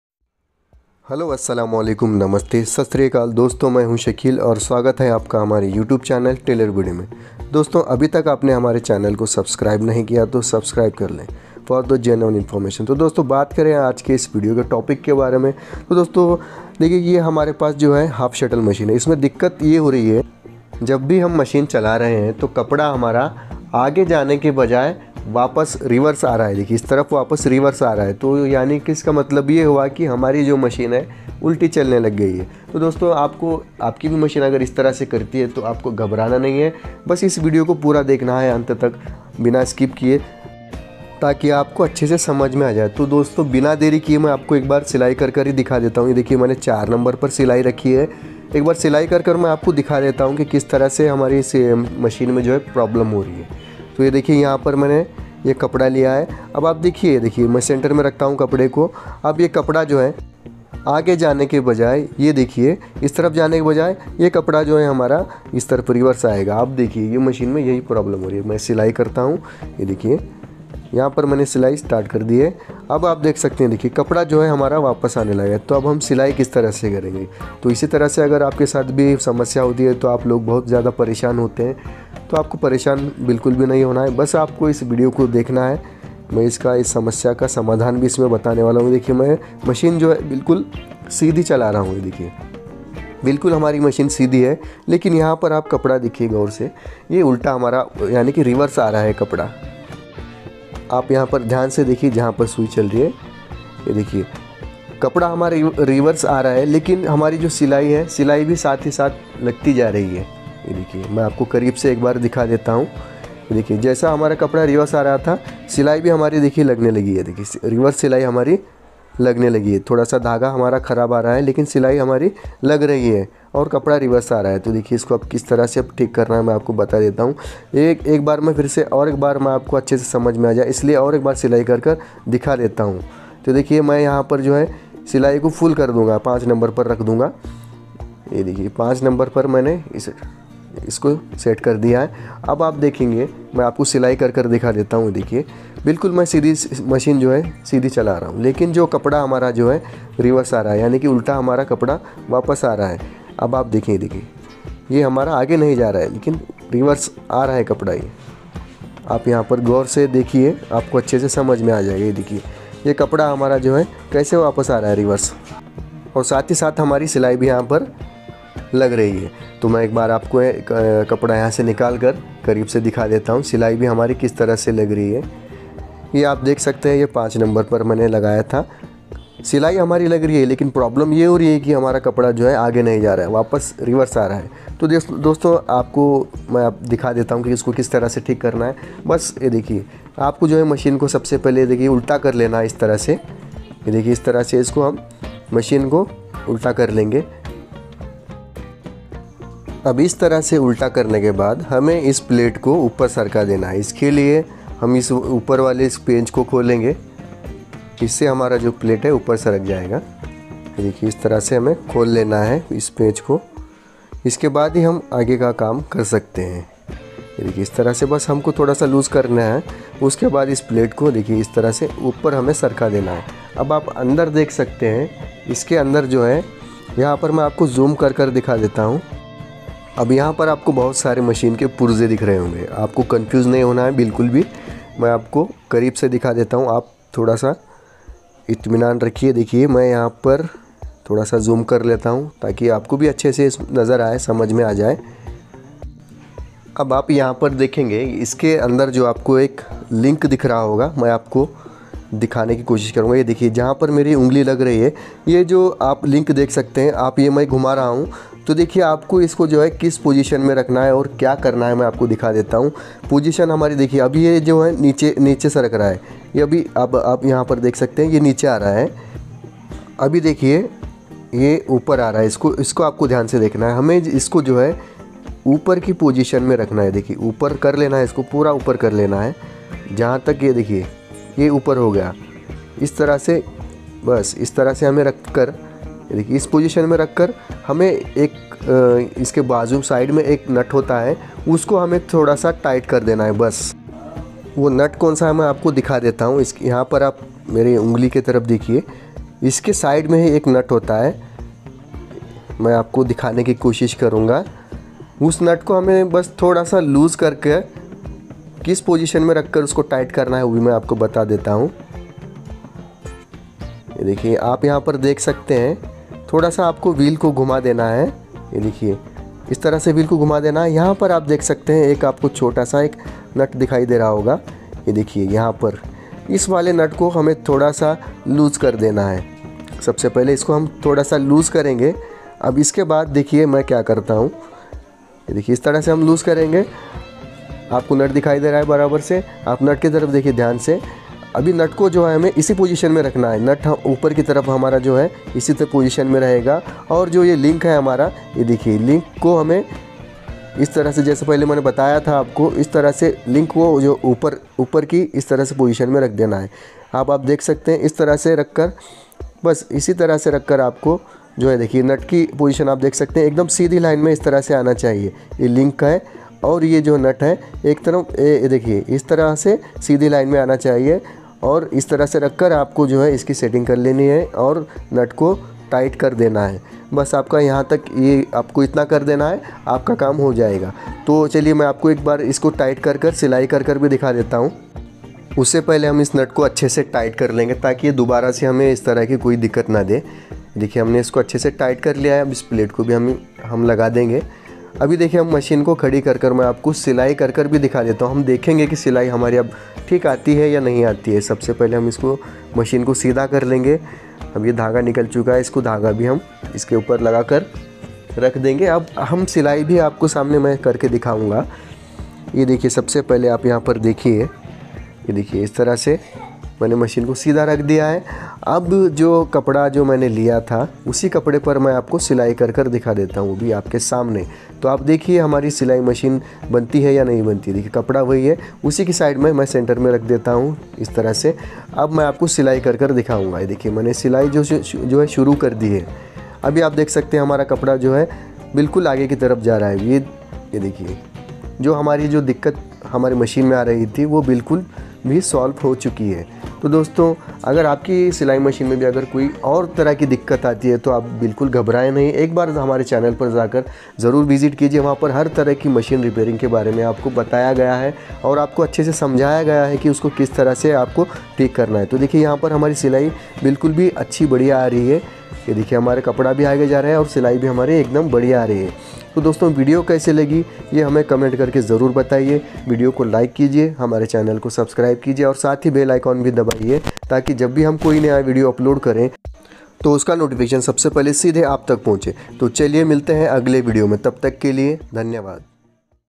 Silai Machine Ulti Kyun Chalti Hai | Silai Machine Ulti Chal Rahi Hai Kya Karen | Tailor Buddy
VIDEO TITLE :- Silai Machine Ulti Kyun Chalti Hai, Silai Machine Ulti Chal Rahi Hai Kya Karen, Tailor Buddy,

Hello friends, agar aap ki silai machine ulti chal rahi hai to aap is video ko shuru se leke end tak pura dekhein, hum aaj ki is video mein aapko machine theek karne ke baare mein batane waale hain. seving theard come not well problem fix. Dhanyavaad. 🙂

MY MOBILE

1 - PERFECT TAILORING SCALE SET

2 - BEST SCISSOR FOR TAILOR CUTTING

3 - CHEAP AND BEST SEWING MOTOR

4 - BEST SEWING MACHINE FOR HOME USE